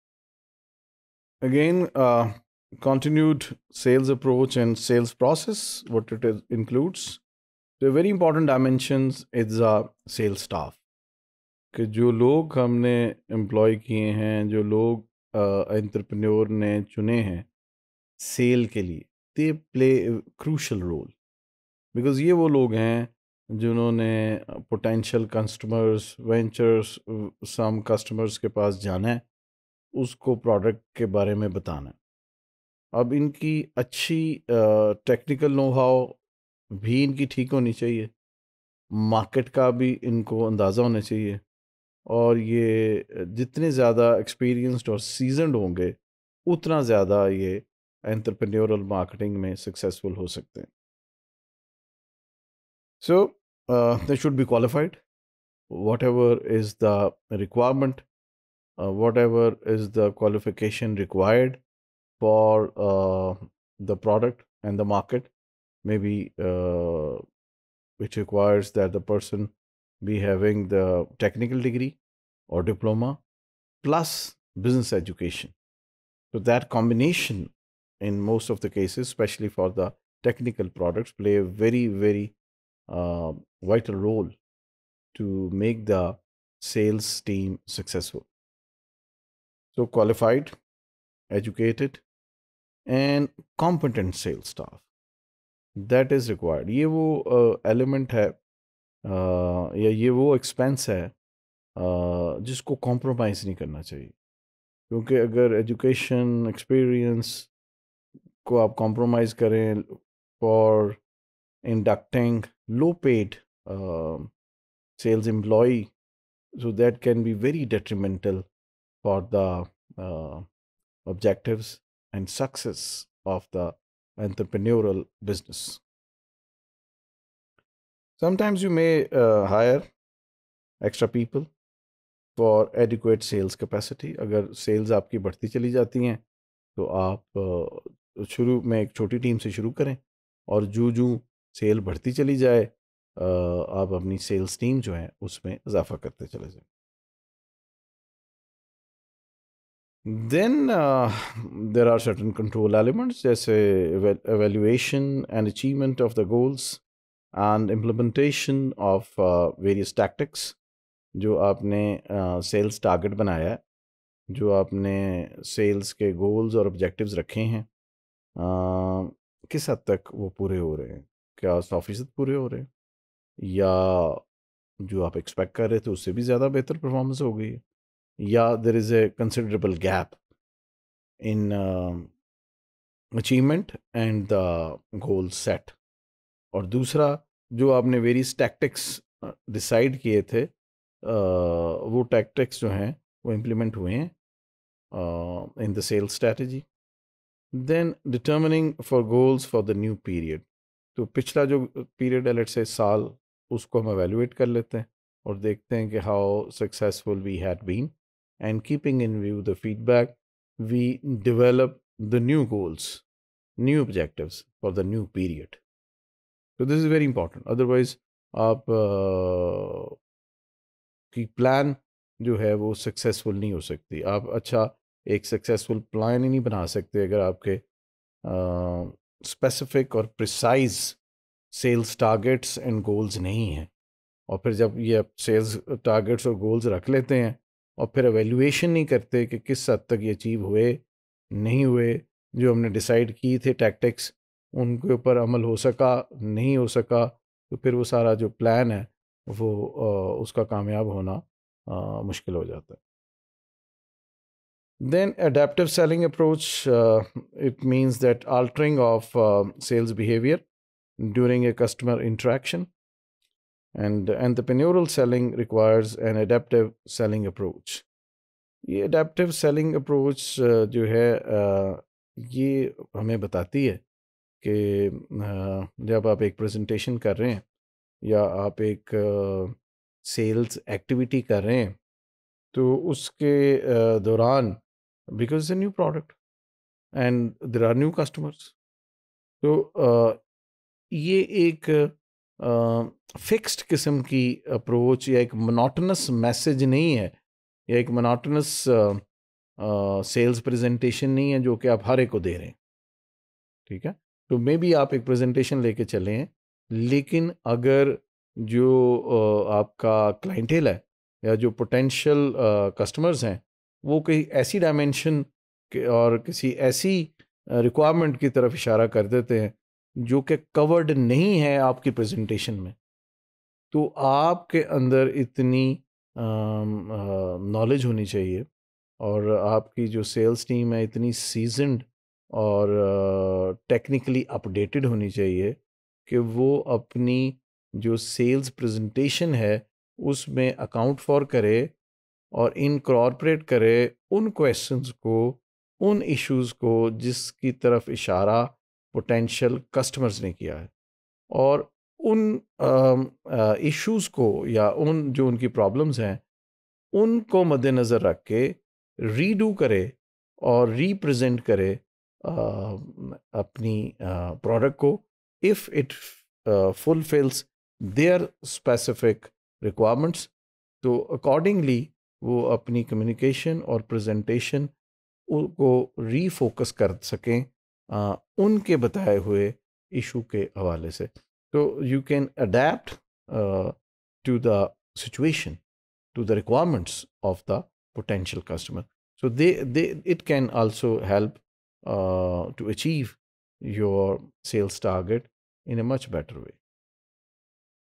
Sales Approach & Sales Process (Cont.) | Entrepreneurial Marketing | MKT740_Topic164
Topic164 - Sales Approach & Sales Process (Cont.)
By Dr. Mubbsher Munawar Khan